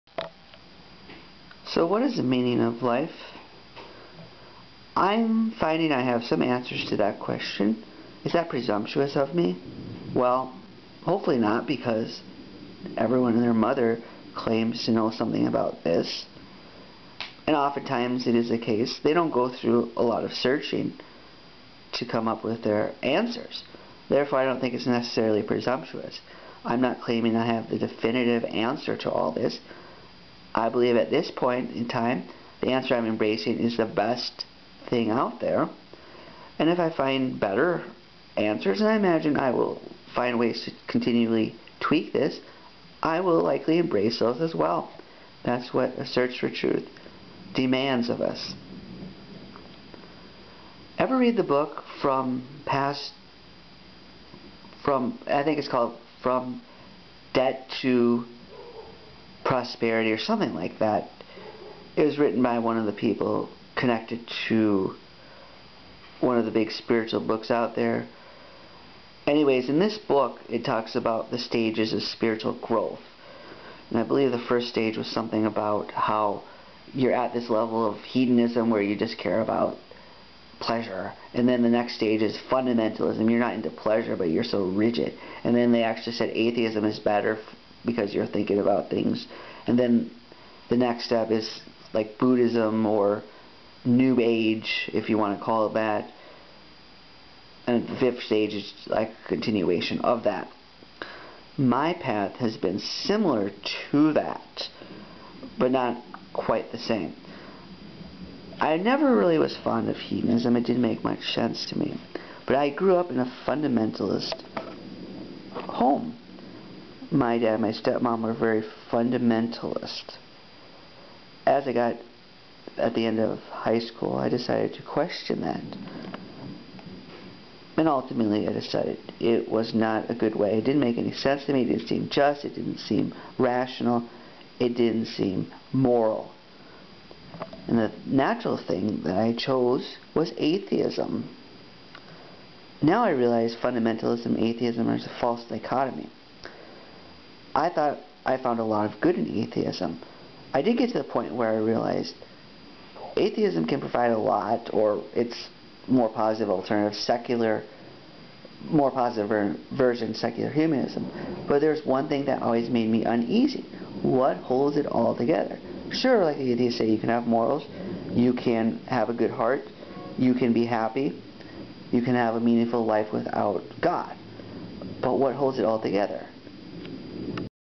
The Meaning of Life Part 1 by Andrew Bushard
Some people say we have alternatives to atheism and fundamentalism.